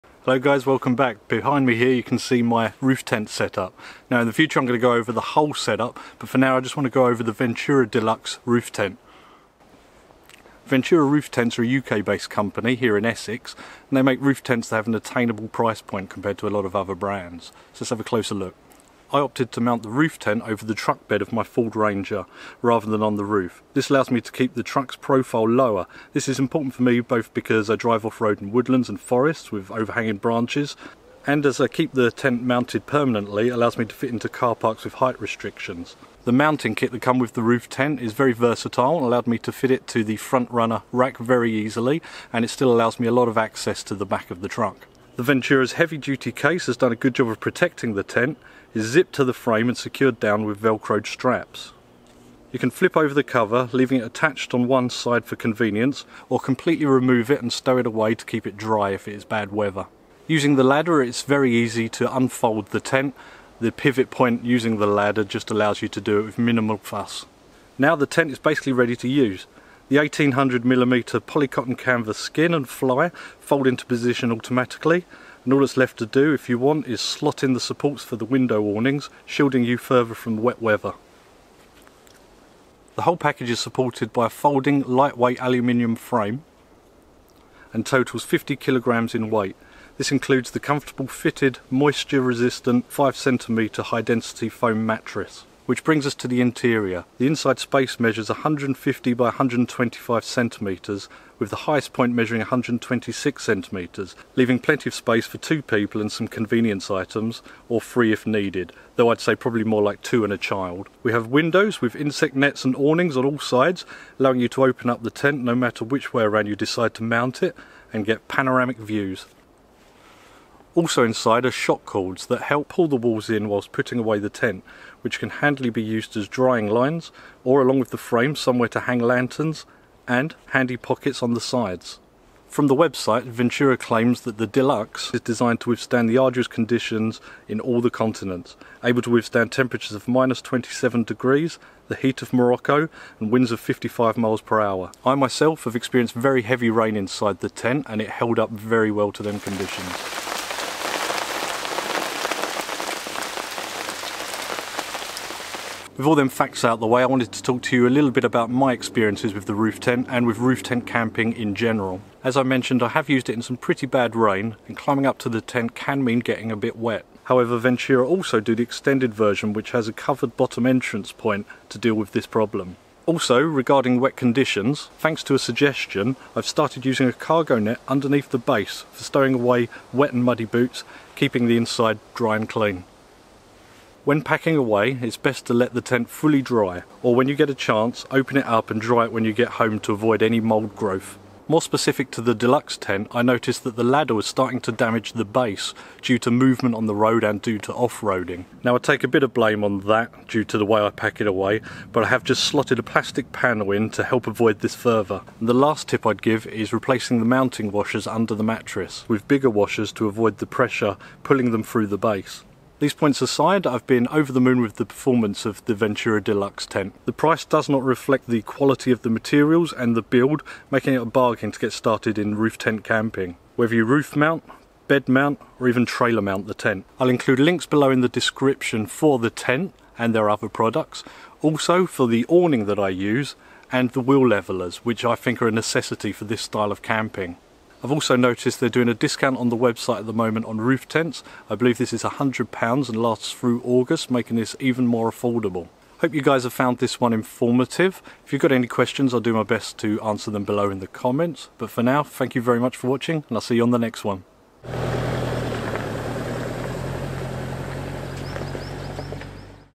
Ventura Deluxe Roof Tent Review - Long Term.
Taking a Closer look at my Truck Roof Tent, the Ventura Deluxe.. perfect for Car / Truck camping.